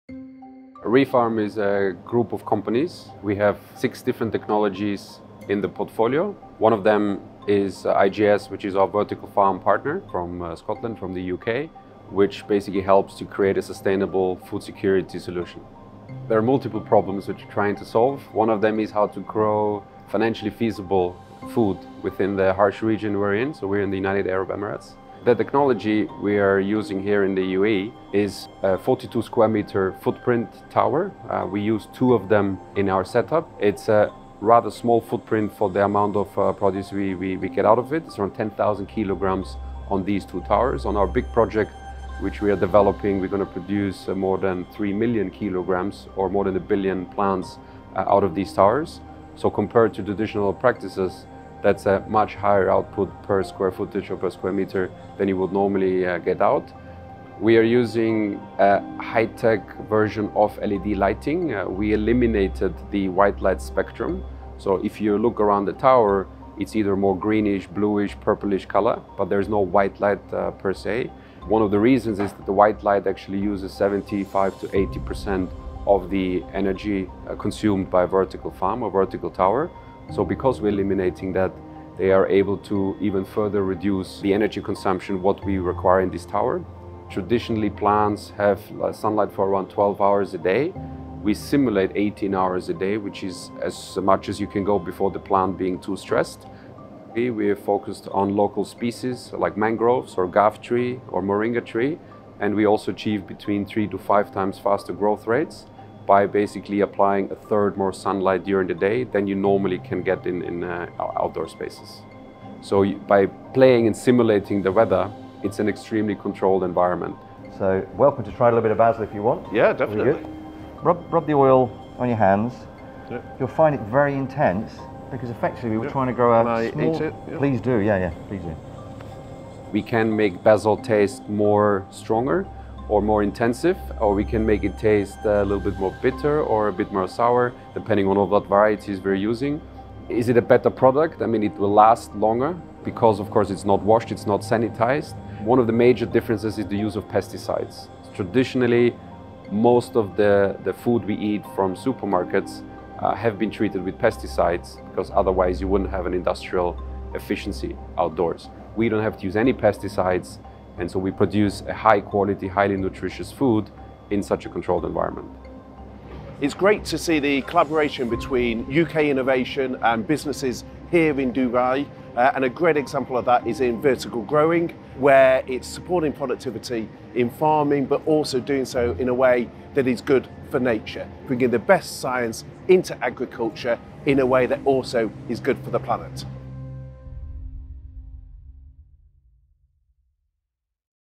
ReFarm, the vertical farm
Have you heard of vertical farming?

Vertical farms are water and energy efficient; help decarbonise food production and strengthen food security. ✅

During COP28 the Environment Secretary visited Intelligent Growth Solutions’ ReFarm, witnessing first-hand how innovation in agri-tech can increase productivity and benefit the environment. 🌍⚙

ReFarm is designed to recycle more than 50,000 tonnes of food waste and grow more than 3 million kg of fresh produce each year!